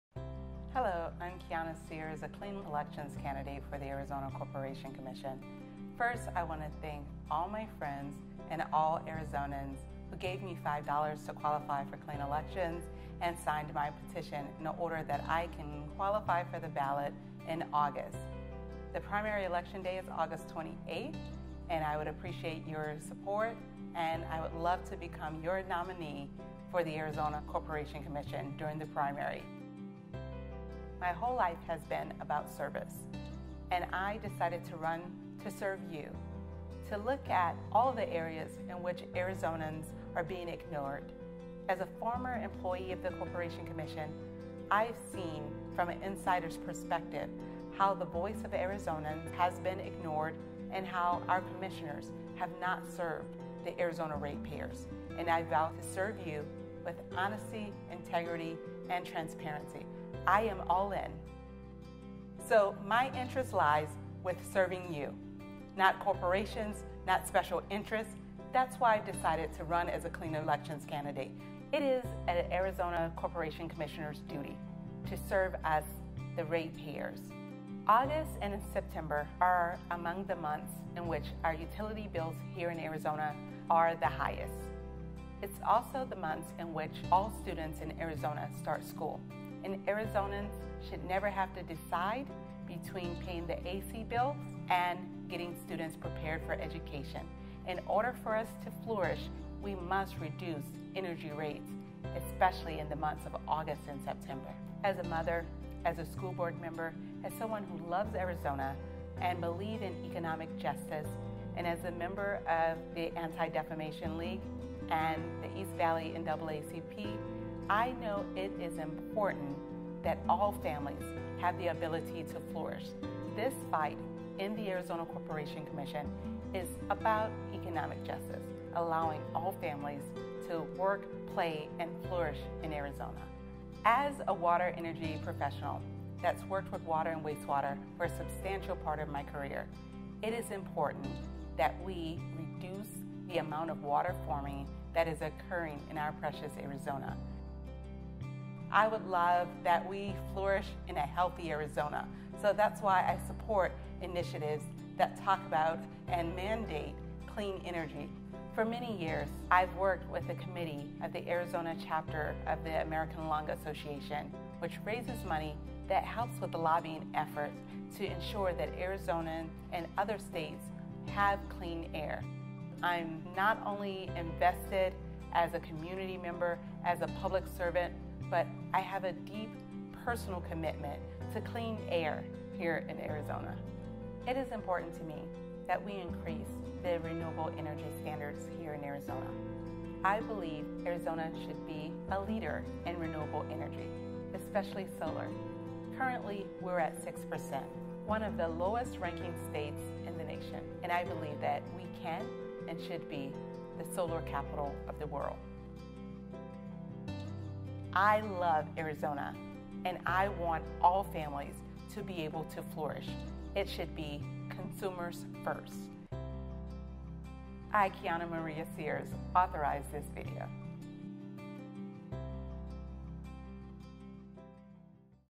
Kiana Sears for Arizona Corporation Commission - Clean Energy, Lower Rates, Transparency
Clean Air, Clean Energy and Clean Politics.

Take action with Kiana Sears. Together we can:
- Bring transparency to the Arizona Corporation Commission,
- Lower your energy costs,
- Conserve natural resources,
- Increase solar availabiltiy
- and Create a brighter future for Arizona.

Kiana Sears for Az Corporation Commission.